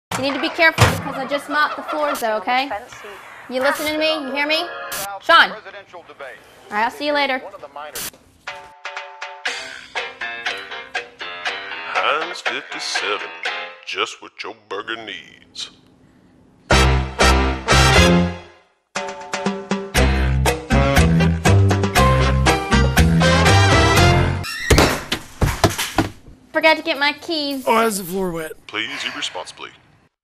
The T.R.U.T.H. Group for Heinz Ketchup
This is our official entry for the Heinz Ketchup Commercial contest.

We FINALLY figured out our fatal mistake!
Like a bunch of goobers, we had the narrator say Heinz 57 instead of Heinz Ketchup. Ha! :D
It's hard to win a contest when you don't promote the right product. ;)

It features Sean Till and his wife, Melissa.
"Please Eat Responsibly"
(Entry #1882, 2007)
We haven't met a woman yet who doesn't LOVE this clip.
We hope you will enjoy it, too.